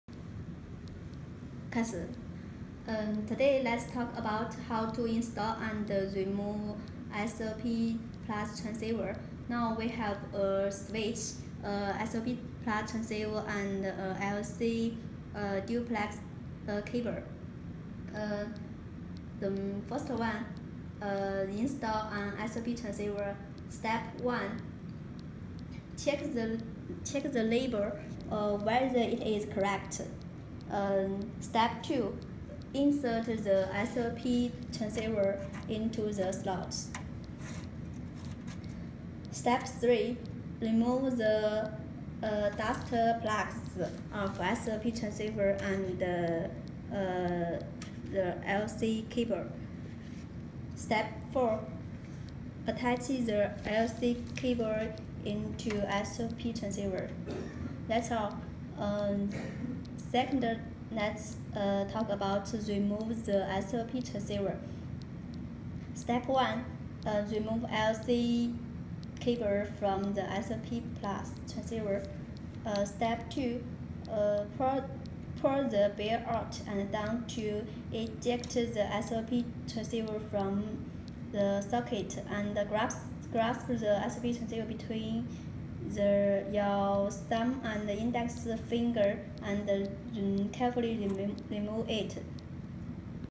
How to install and remove 10G SFP+ SR Modules?
This video mainly introduce installing and removing of SFP+ transceiver.We also introduce some common questions and precautions about before and after purchase this product. Click the following link to see more details